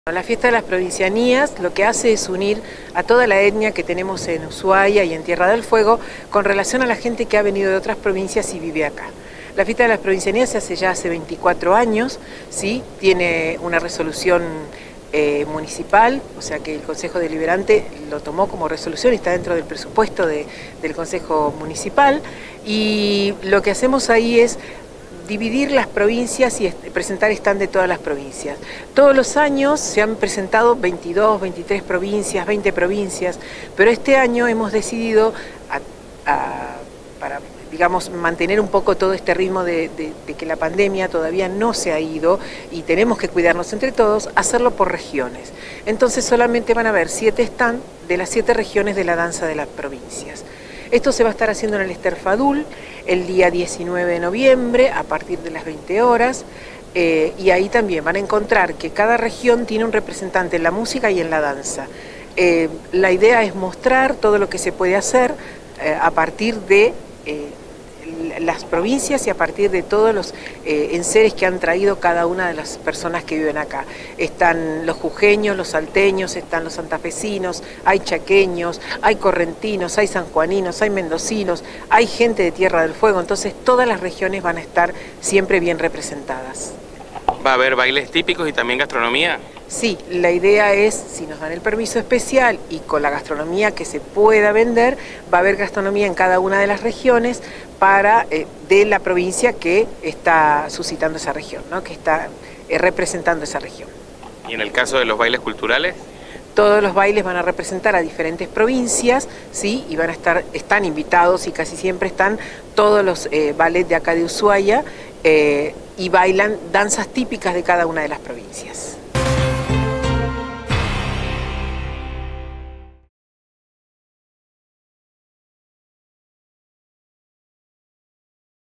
FIESTA DE LAS PROVINCIAS EN USHUAIA.
Será el próximo 19 de noviembre con comidas y bailes típicos de cada región del país